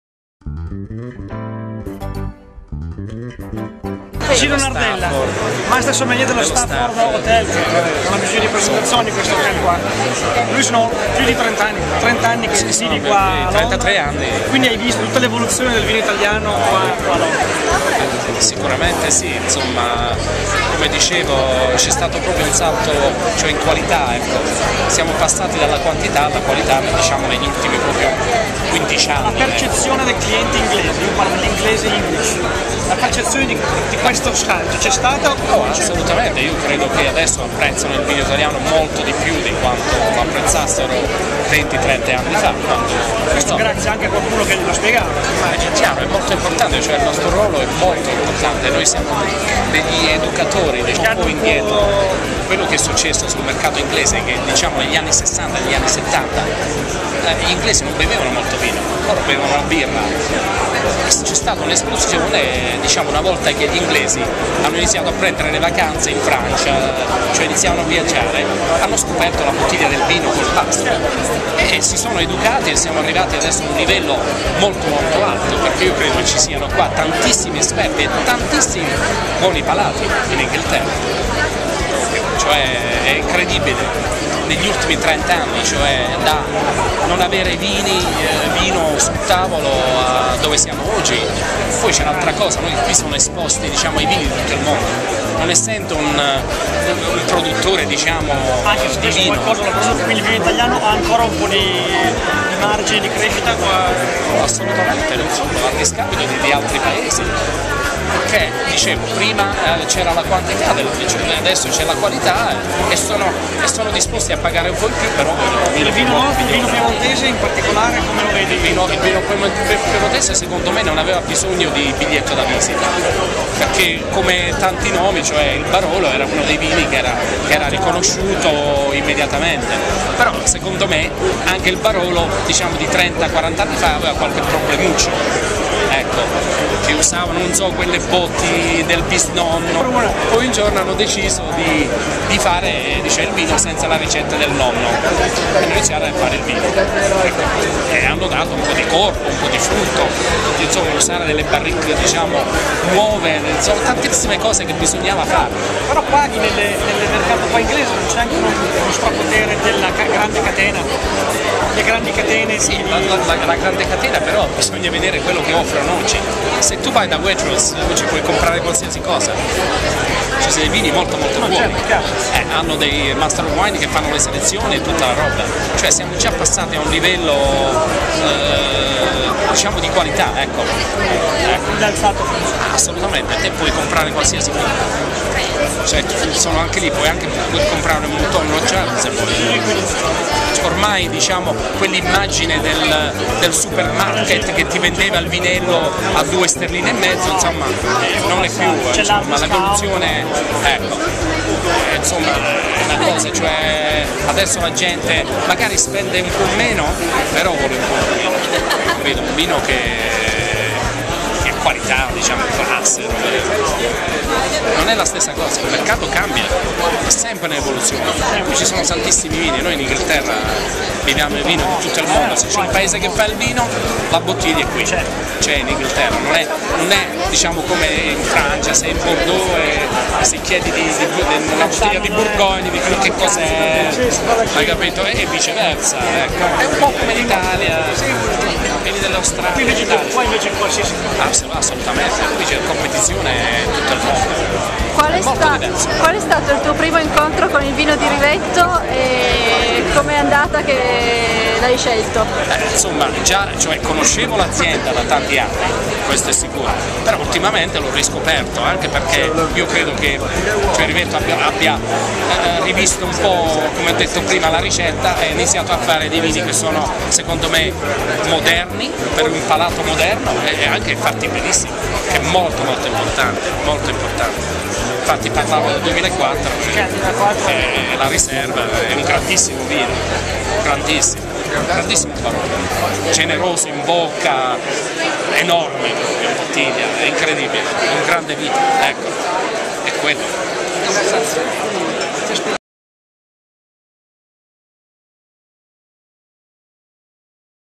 Rivetto a Londra - Intervista a Nardella.mpg
In occasione del Definitive Italian Wine Tasting Enrico Rivetto intervista sommelier importanti per il mercato del vino italiano